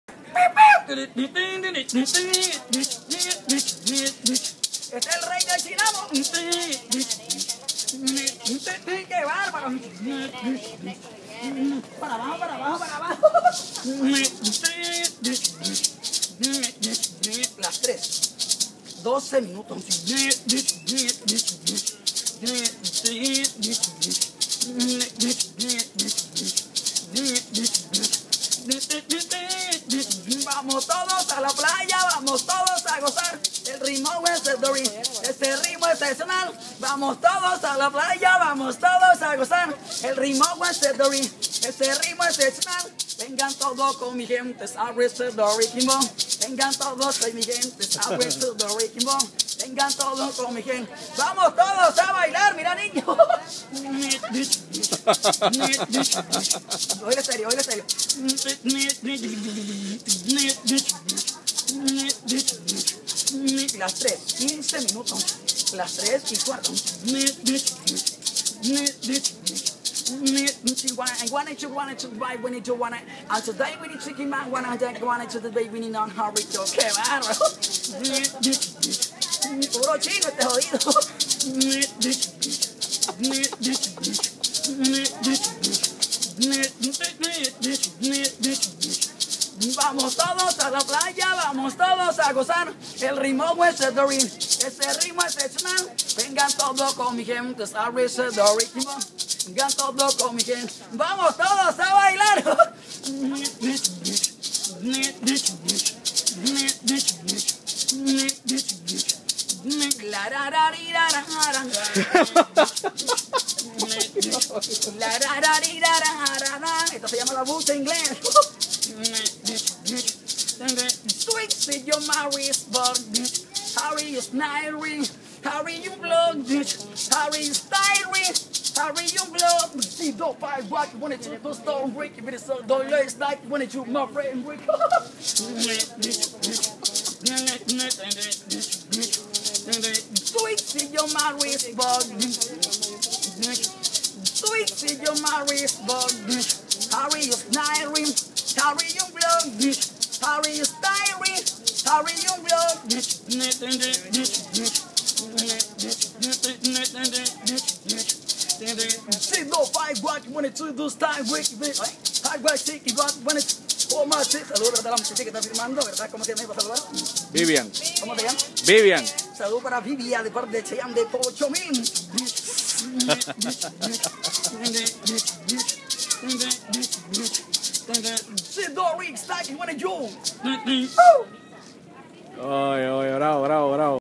El chayanne de pochomil
El chayanne de pochomil le canta y le baila a la Vivian Peng